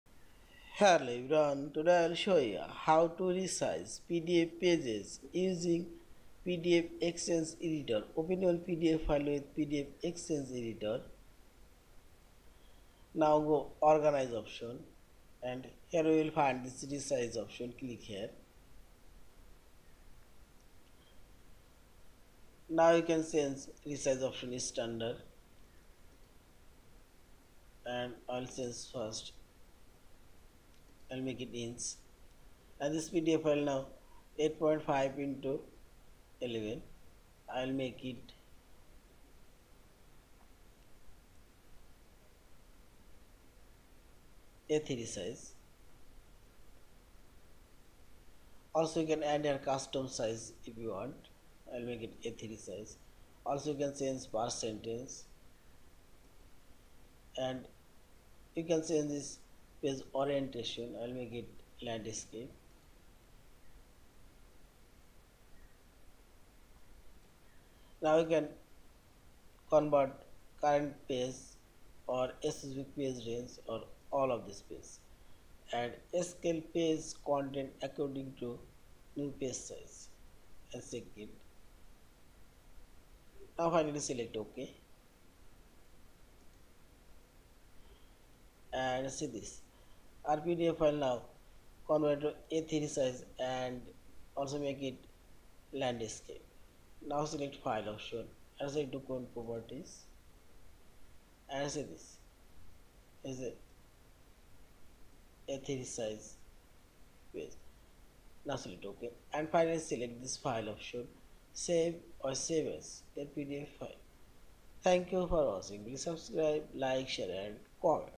How to resize pdf pages in PDF-XChange Editor
Hello everyone, Today I will show you, How to resize pdf pages in PDF-XChange Editor.